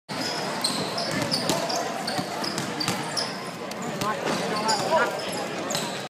Matt vinci wabl 2013
Matt Vinci dunking a basketball with a 40 inch vertical